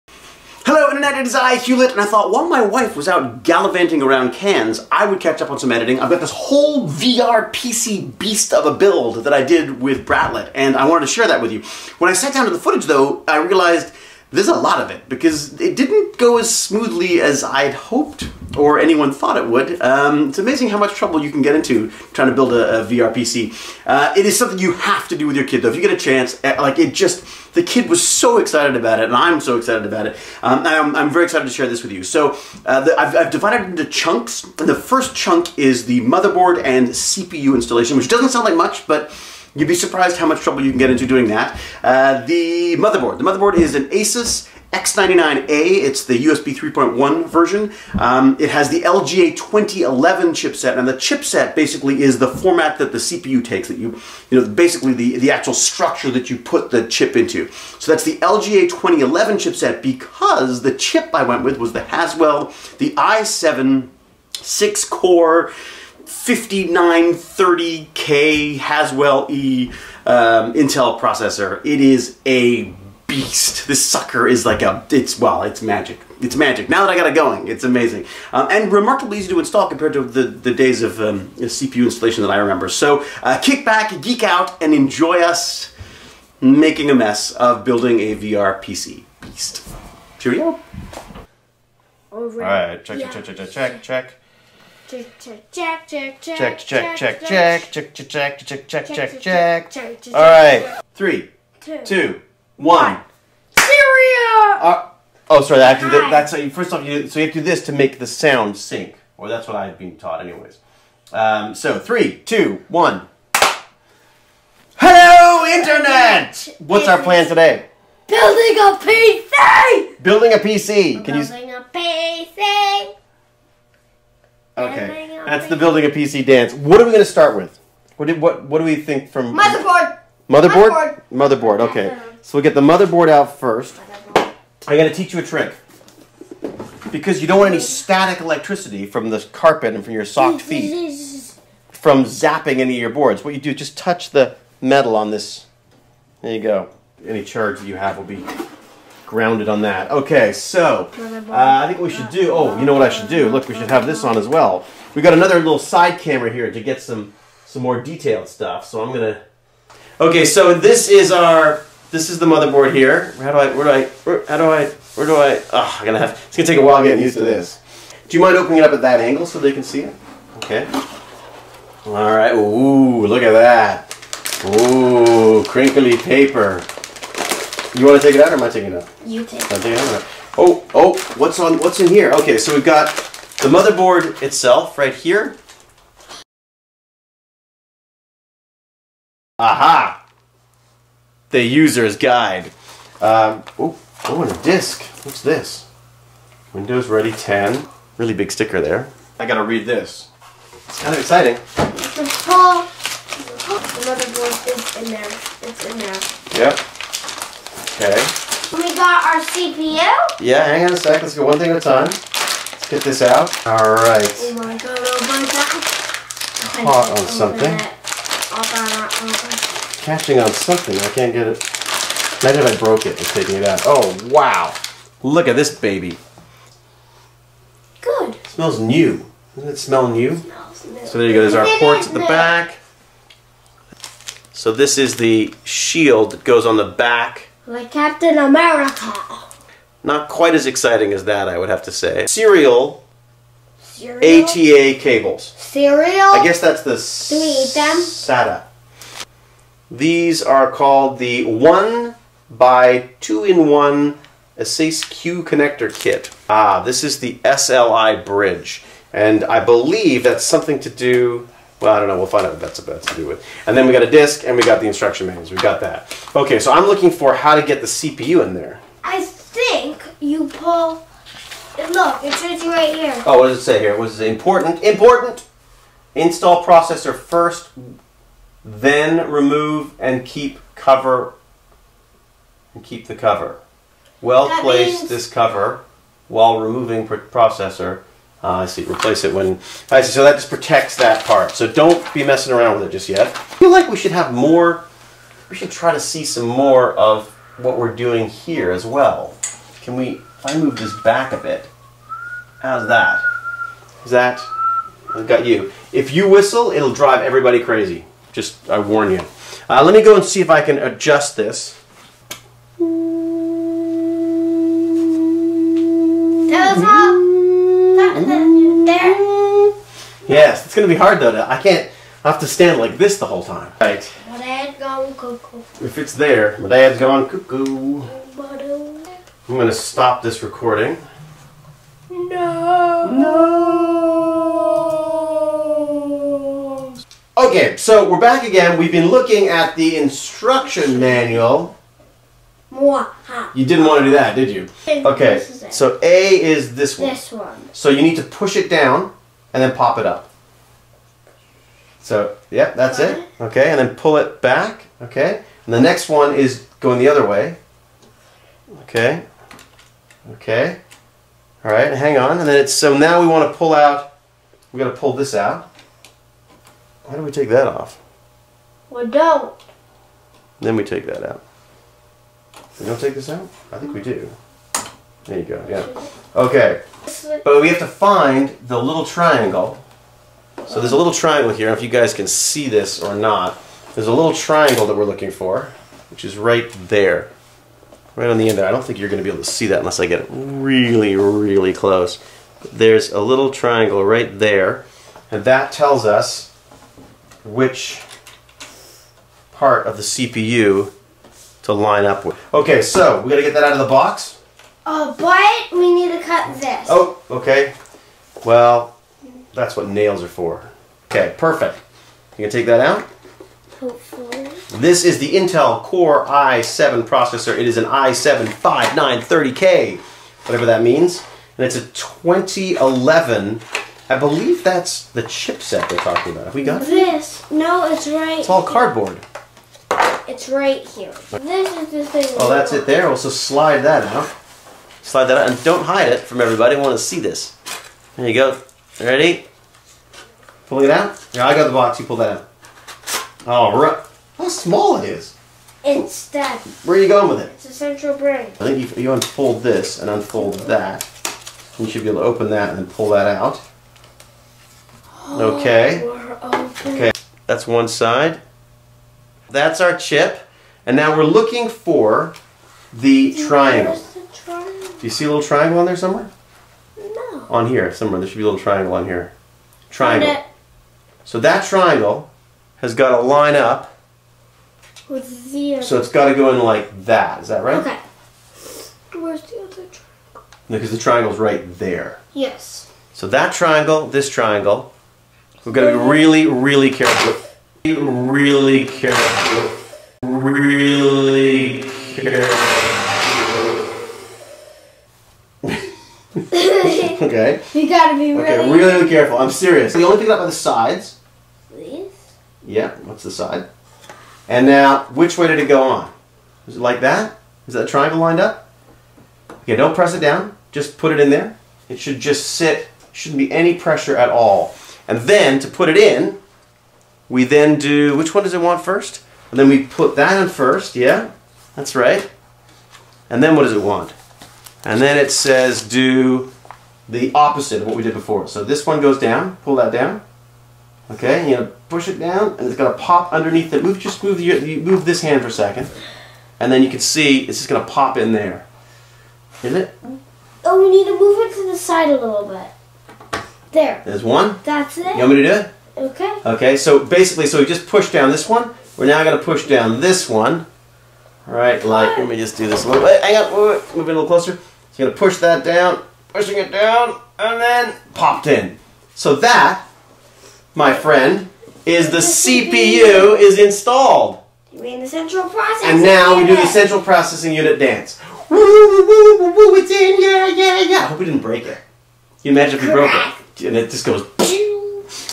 VR PC Beast build - Part 1
Sure, I can fix a Stargate, but have a look at what a mess Bratlett and I can get into when we set about to build our VR PC!  This is the first part of our Geek and Son VR PC build series and sees us unboxing and installing the ASUS X99-A motherboard and the glorious Intel i7- 5930K Haswell -E 6 core 3.5 GHZ LGA2011 processor!  Huge thanks to PCPerspectives for all their help and guidance in these matters!